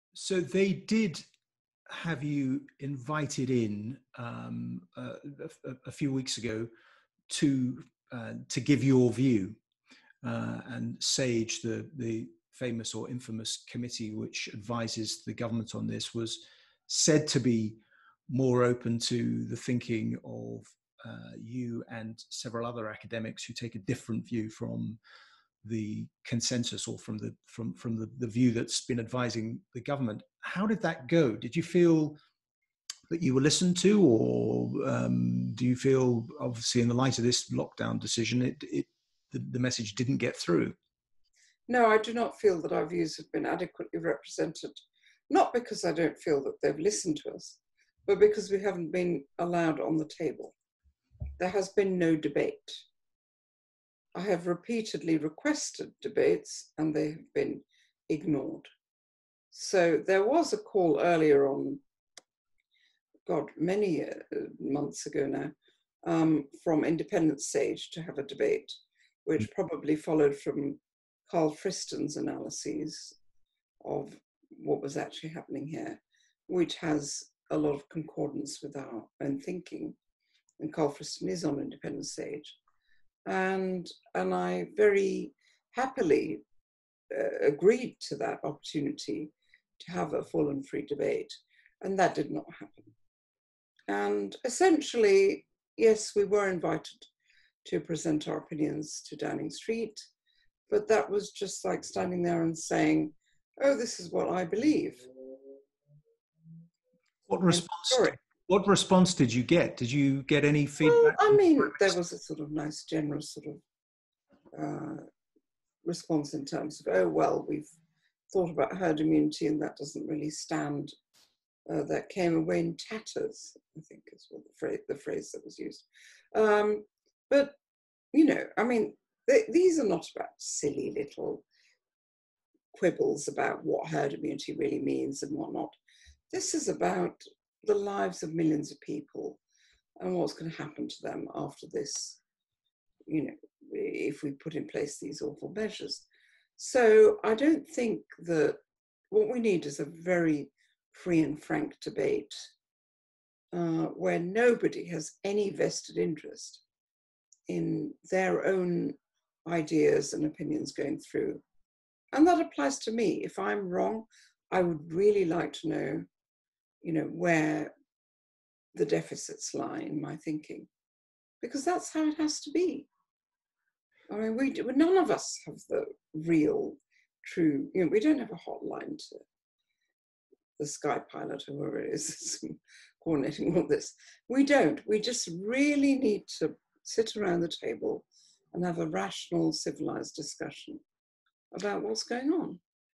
Professor Sunetra Gupta: "There has been no debate"
Professor Sunetra Gupta, leading lockdown sceptic, interviewed by Reaction editor Iain Martin.